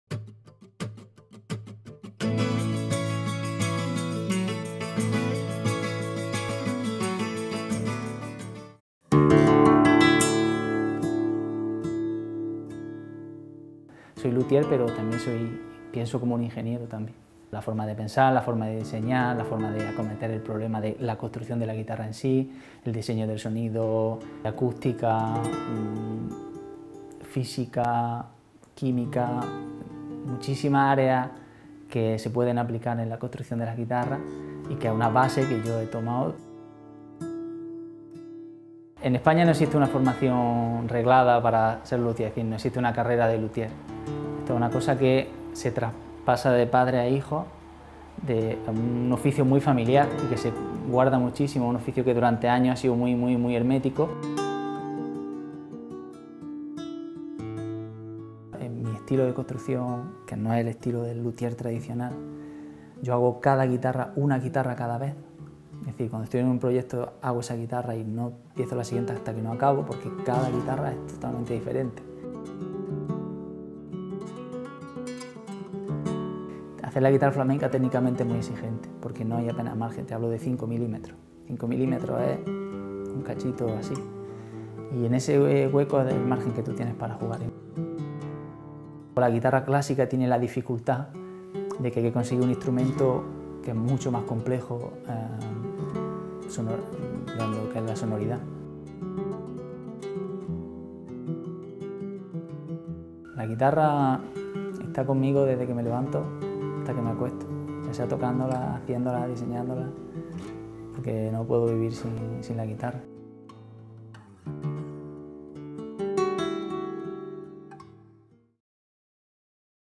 HDL Paco Chorobo, de ingeniero a lutier cuyas guitarras suenan hasta en Nepal
Paco Chorobo (Úbeda, Jaén) dejó su trabajo como ingeniero de telecomunicaciones en una multinacional para centrarse en su pasión: ser lutier. Empezó en 2012, construyendo una guitarra para un amigo y desde entonces ha realizado casi medio centenar. Mantiene su carrera como guitarrista y se ha iniciado en la docencia, impartiendo cursos de construcción. Cada guitarra que hace es un proyecto personalizado, a medida del guitarrista. Esa filosofía lo ha puesto en contacto con intérpretes de medio mundo: de Chile a Nepal o Irán, de China a Suiza.